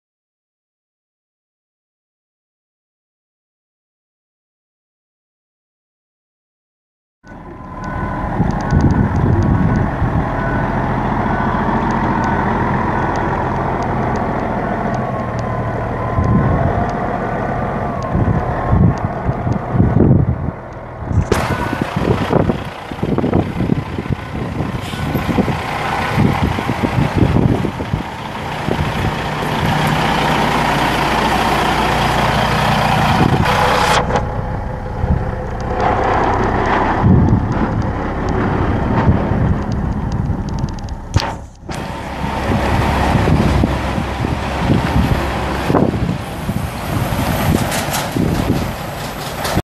1996 Peterbilt 379 semi truck for sale | no-reserve Internet auction September 22, 2016
1996 Peterbilt 379 semi truck
1,003,912 miles on odometer
Tandem axle
Tilt
Telescoping steering wheel
Cruise
AM/FM/cassette
Passenger power window
Heated mirrors
Cobra 29NW LTD Classic CB
Backup alarm
Block heater
Dump valves
PTO
Wet kit
Chain hanger
Storage
Mud flaps
212" wheelbase
11R24.5 tires
17,065 lbs chassis weight
Unit# 880
1XP5DB9X8TD403130

1XP5DB9X8TD403130

Location
1021 S 13th St
Tekamah, NE 68061

Item location address: This item is located at 1021 S 13th St, Tekamah, NE

It's located in Tekamah, NE, and we're selling it to the highest bidder regardless of price.